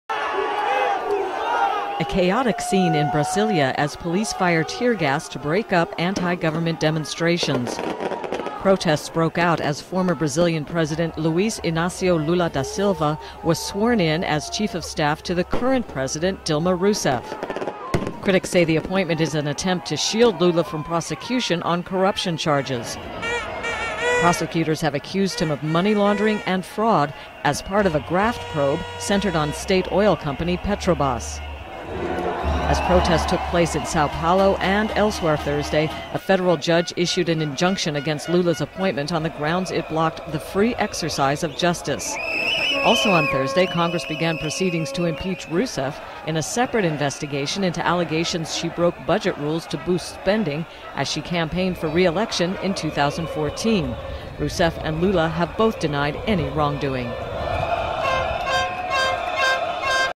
Brazilian Police Fire Tear Gas On Demonstration Against Lula Assuming Ministry
March 17, 2016 - Police use tear gas on protesters angered after Brazil's ex-president, who is facing corruption charges, is sworn in as chief of staff to the current president. Katie Sargent reports.